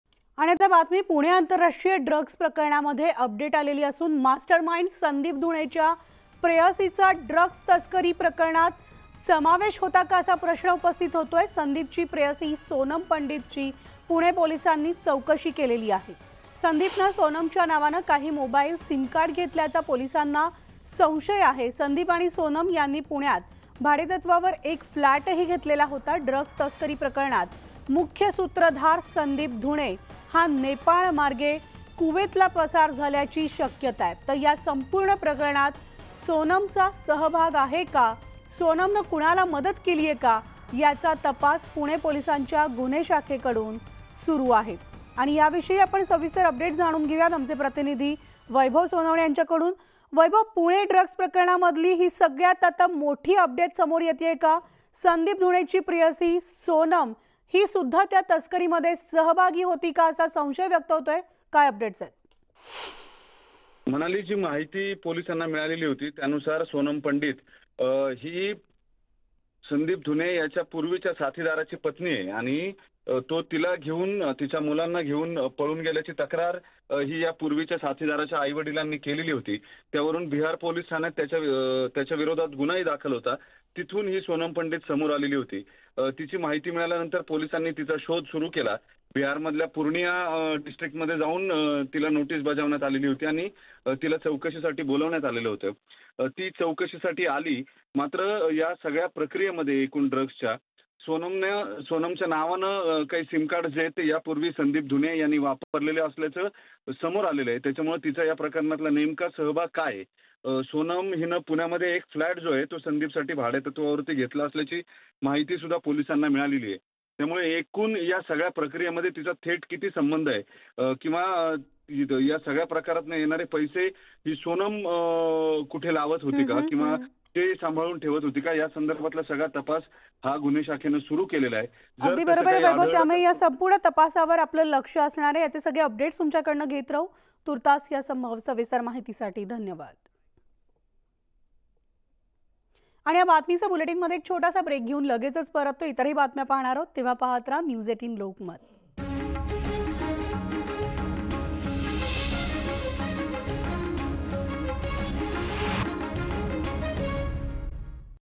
Pune Drug Case : संदीप धुणेची प्रेयसीही तस्करीत सहभागी? | Sandeep Dhunia
Producer / Editor / Publishing : Mandar Rajapurkar